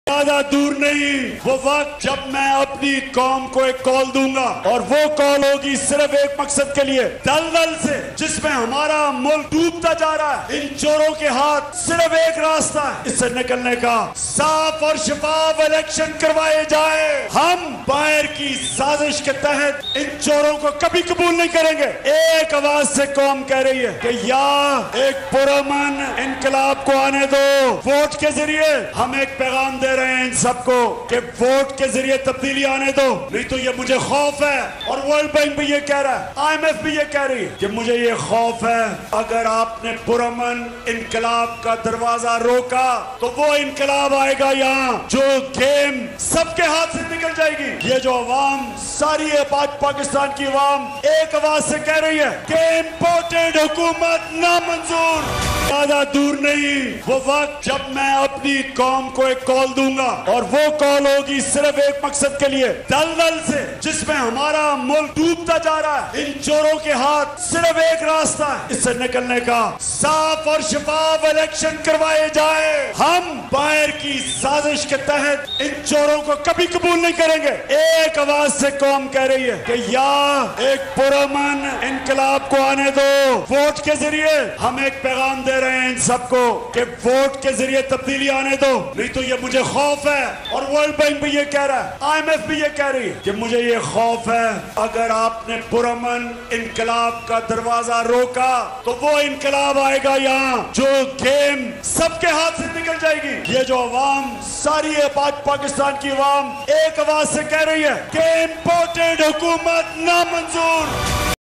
چارسدہ جلسہ || کپتان کا جارحانہ خطاب || آج ساری قوم کا ایک ہی نعرہ ہے امپورٹڈ حکومت نامنظور
چارسدہ جلسہ
عمران خان کا خطاب
آج ساری قوم کا ایک ہی نعرہ ہے امپورٹڈ حکومت نامنظور
آج ساری قوم کا ایک ہی مطالبہ ہے صاف اور شفاف انتخابات کرائیں
معاملات وہاں تک نہ لے جائیں کے گیم سب کے ہاتھ سے نکل جائے